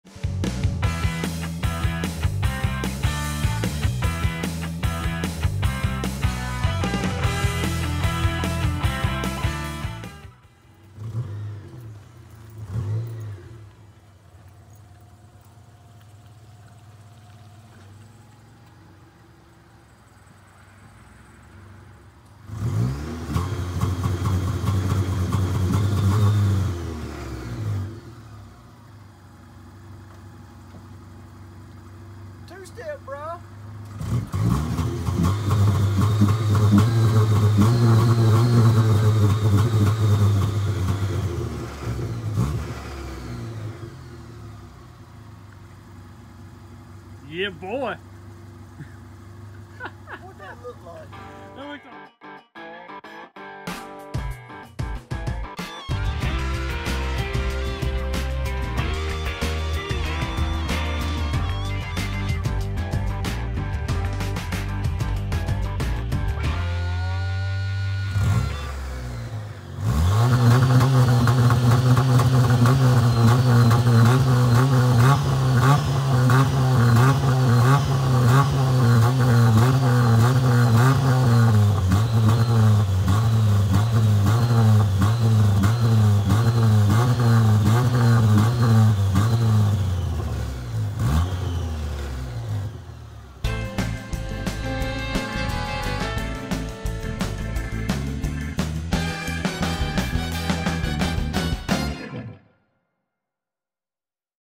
Tuning the Festy?!?!
Well guys just a short video today. I had just dropped my brother off at his house when he captured this! We was doing some fine tuning on the Ol Festy! lol Is it running a bit rich? Is it 2-step? Is it anti-lag? Or maybe it's my anti-tailgating device!! Either way it looks really good at night! What do you think?